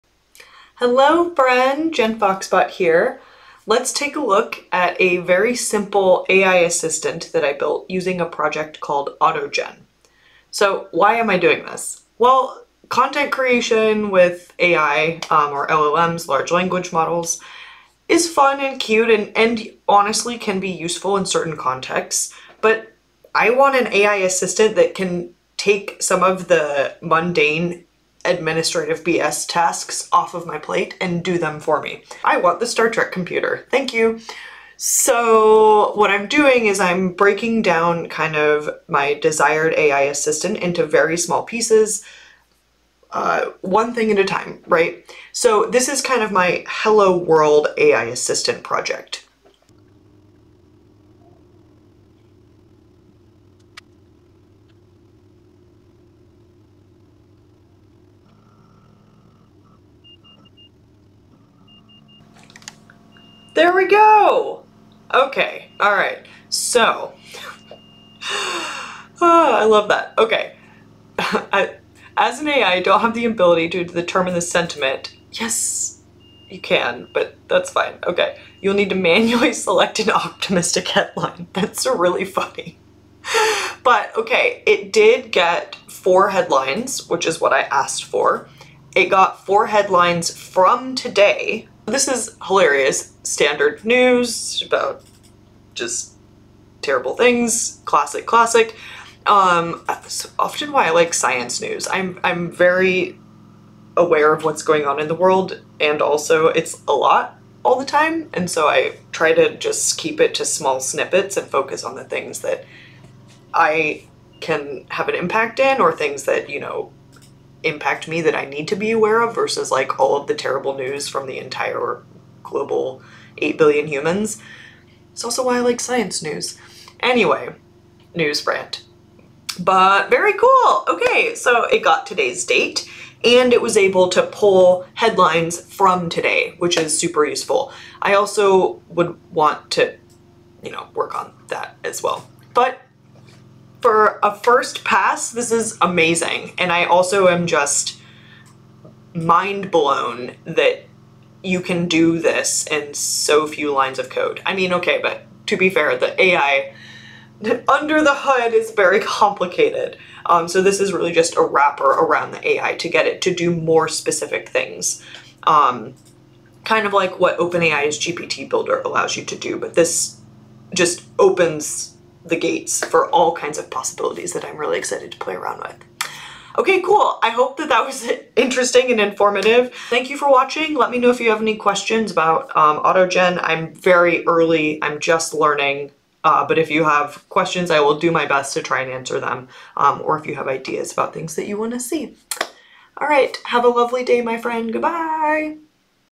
AI Assistant Short Demo!
a "get to the point" demo of my AI assistant made with AutoGen!